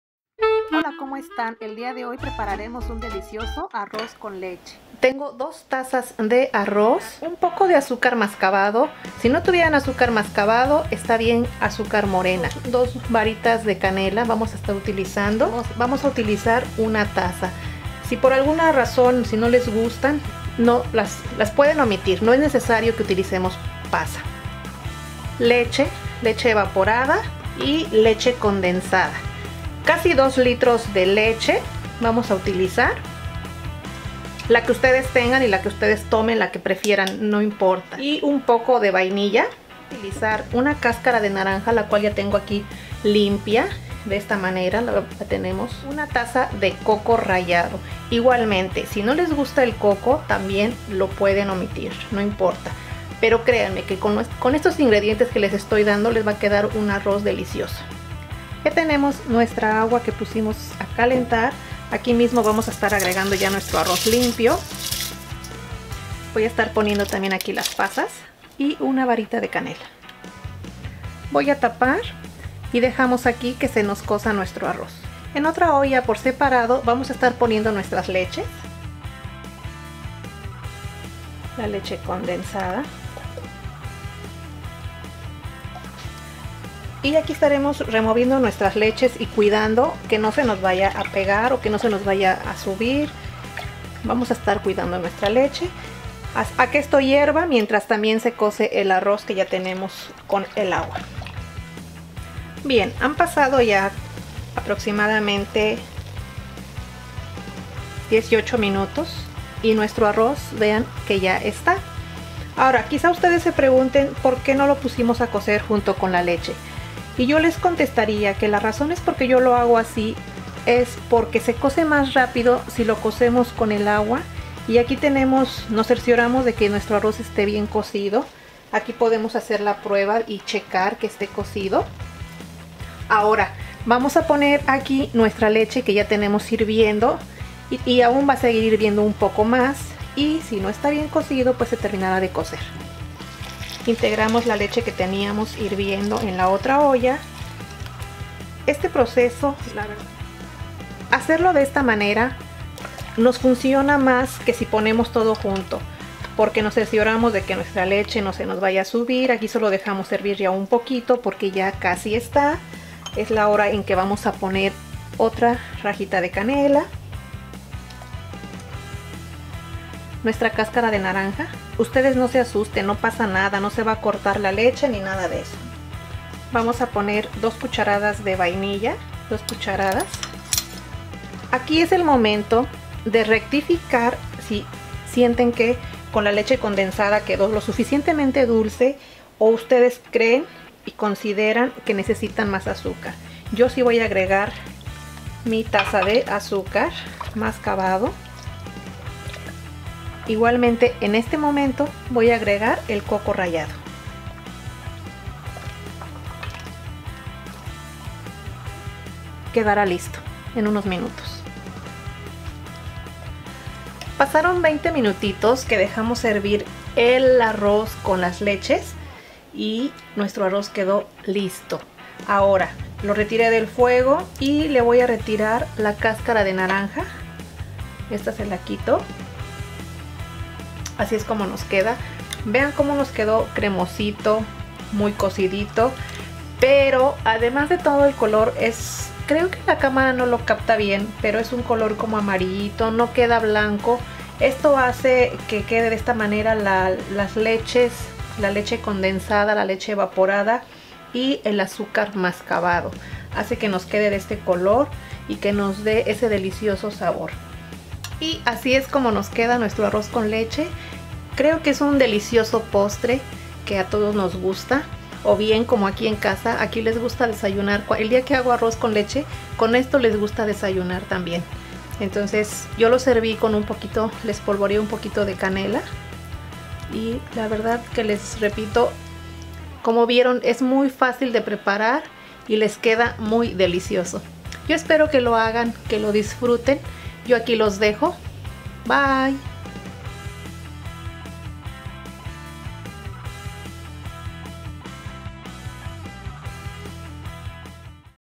ARROZ CON LECHE CREMOSO...!!! | JOSS YUMMIE
TE INVITO A MI OTRO CANAL   JOSS BEAUTY...!!!

INGREDIENTES:

_ 2 TAZAS DE ARROZ
_ 1 TAZA DE AZUCAR
_ 1 TAZA DE PASAS
_ EXTRACTO DE VAINILLA
_ COCO RAYADO
_ CASCARA DE NARANJA
_ 2 LITROS DE LECHE ENTERA